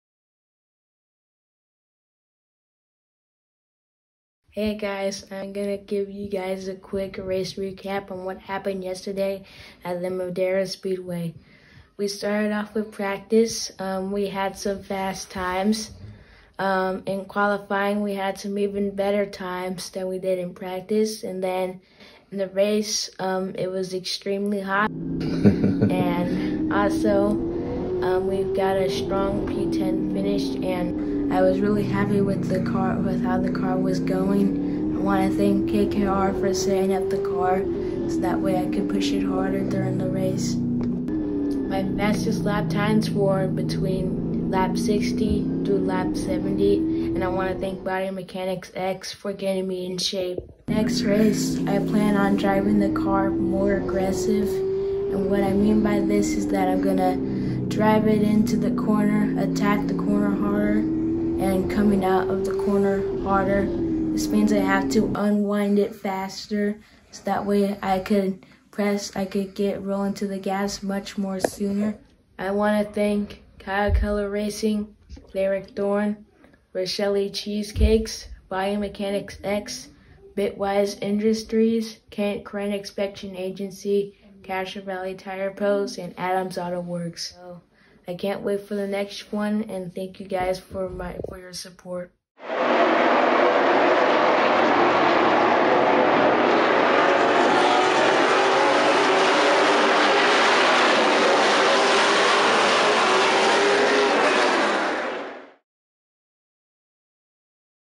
2nd Madera Race
Here’s the recap for this weekend’s race at Dixon. And the dirt continues to be unkind 😒 Hopefully we’ll get some racing luck soon 🤞🏾runcmcracing